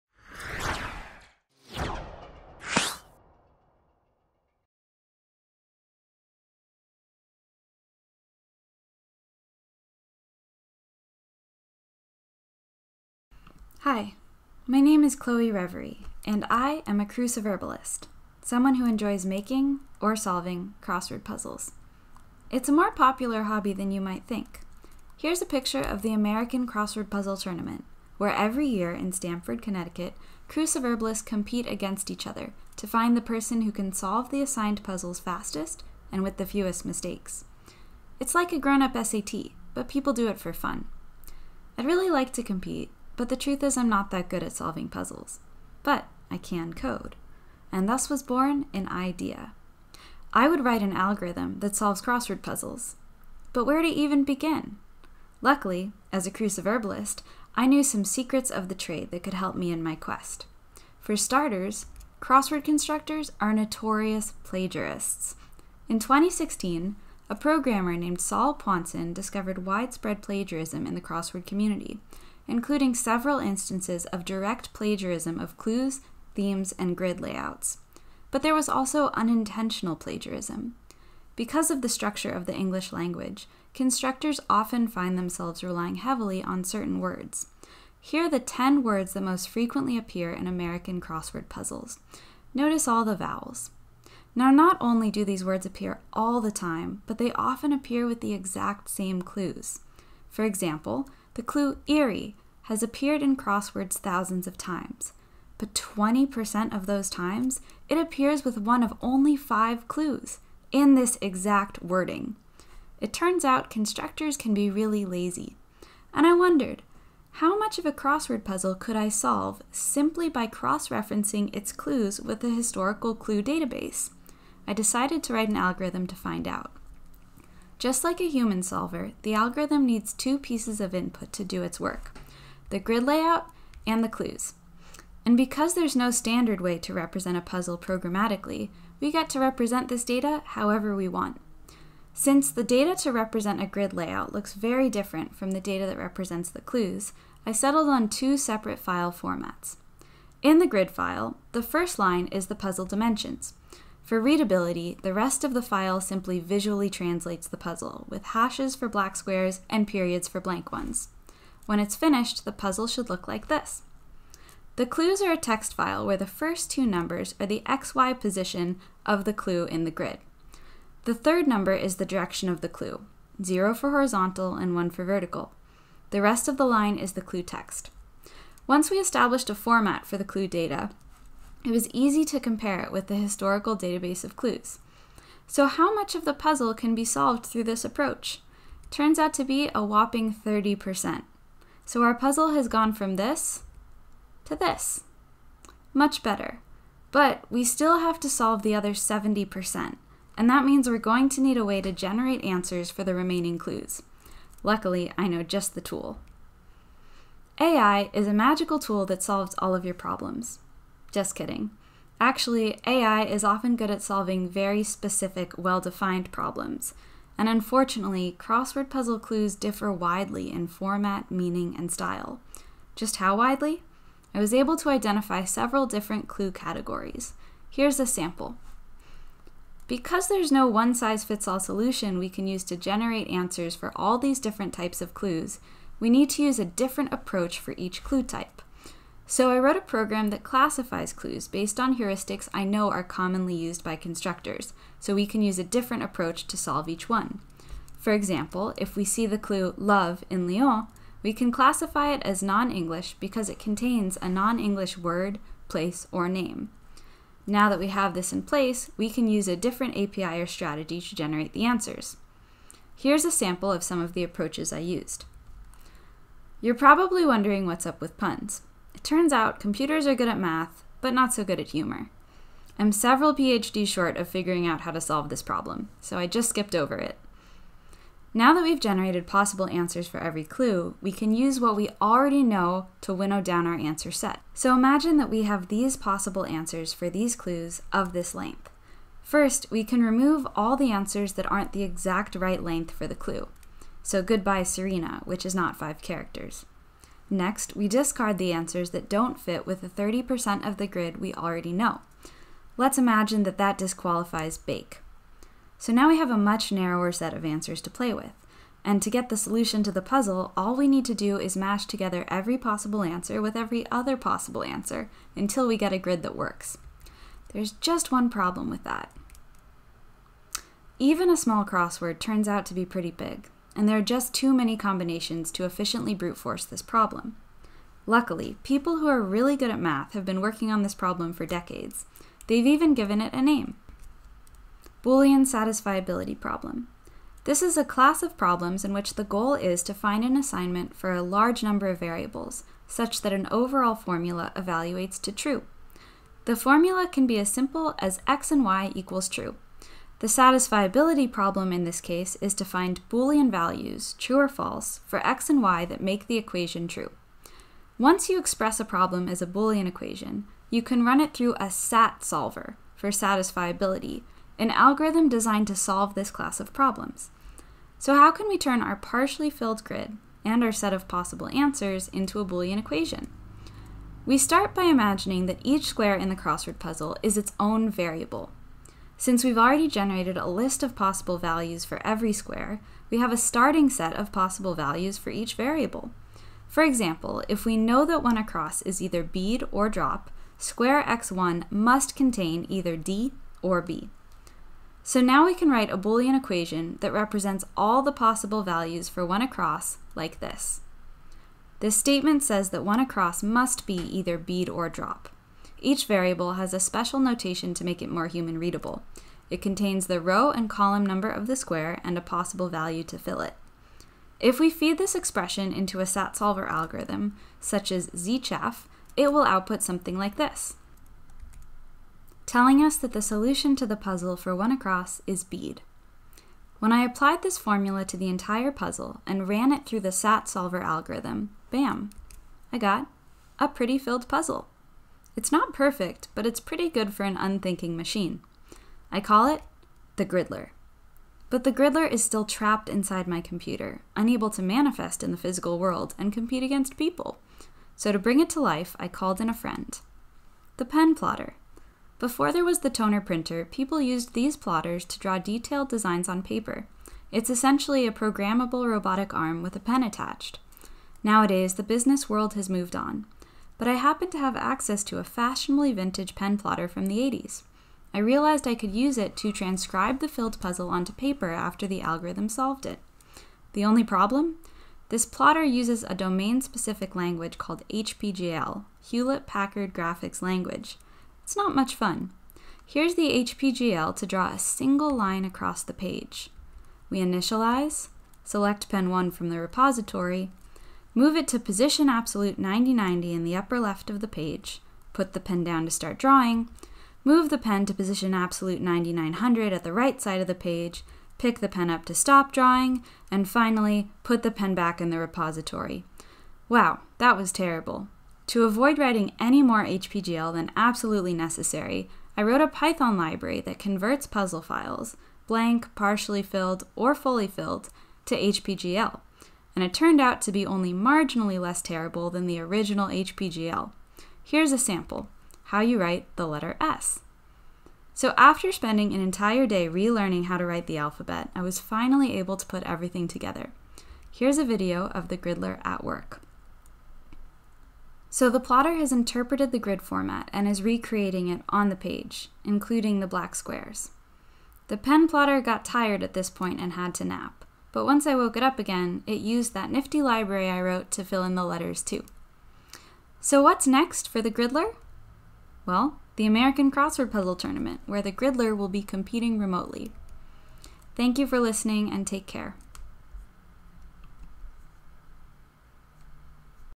!!Con 2020 - The Taming of the Clue: Making a Crossword Solver Bot by Chloe Revery
The Taming of the Clue: Making a Crossword Solver Bot by Chloe Revery

Have you ever tried to solve a crossword puzzle and walked away stumped? Imagine being a secondhand pen plotter from the 1980s! Together we’ll equip it with a shiny new algorithm that parses and solves modern crossword puzzles. On the way, we’ll invent a new file format (or two!), teach the plotter to write the alphabet, and learn about a plagiarism scandal that rocked the crossword world. In the grand finale, the plotter will go head-to-microchip with some of the best solvers in the U.S. in a race against the machine.

Chloe Revery is a San Francisco based coder who makes programs, puzzles, and puns.